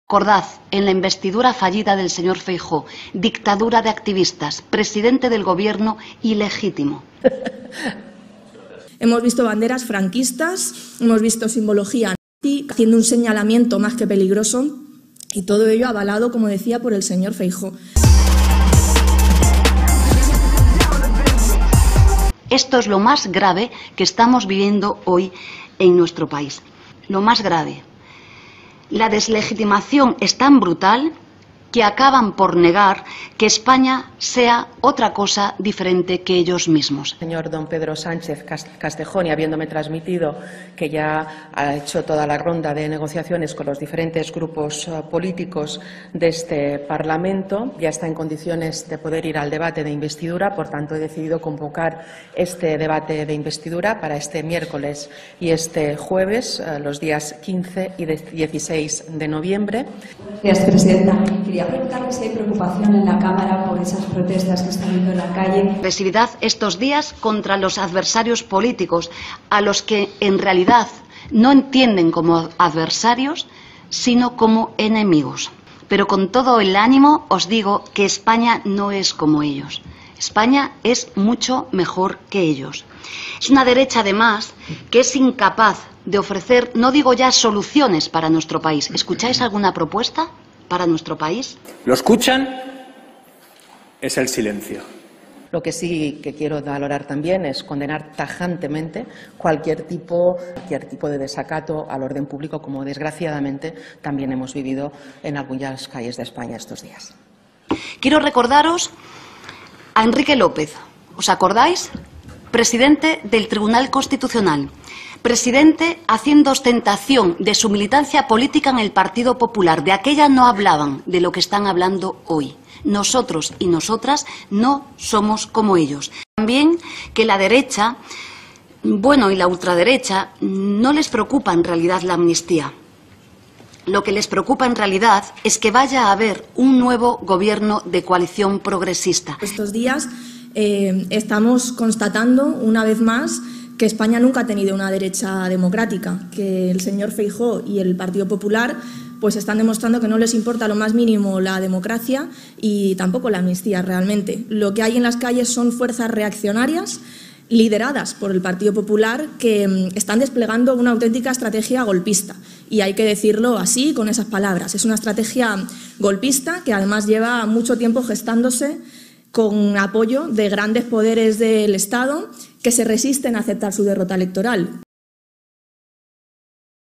🔻La IZQUIERDA en TROMBA, RESPONDE a los NOSTÁLGICOS de la oposición 💪
La presidenta del Congreso de los Diputados Francina Armengol, ha anunciado que la sesión de investidura del Presidente Pedro Sánchez, será esta semana, los días 15 y 16.
Tras el anuncio Teresa Pérez ha comparecido en rueda de prensa para catalogar a los manifestantes que se han podido ver estos días en Ferraz y en sol.
También la ministra de Trabajo, Yolanda Díaz, ha querido compartir su opinión sobre estos manifestantes.
🔴 El lado izquierdo es un canal de debate, opinión y actualidad, donde explicamos y desmentimos falsas noticias en España.